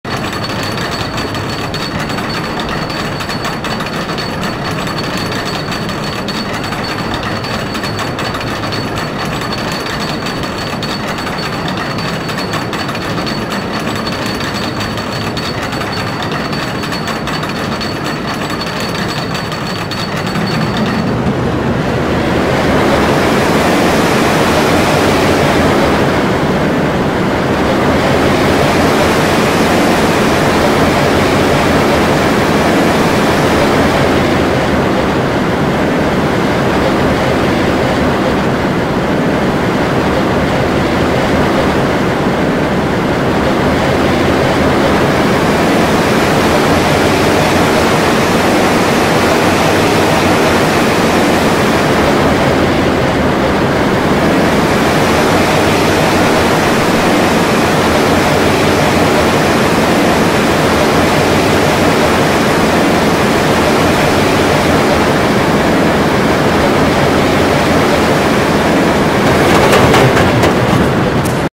Wildcat — A fast-paced terrain woodie — Made With FVD++
Drop: ~145ft
Ride Time: 51s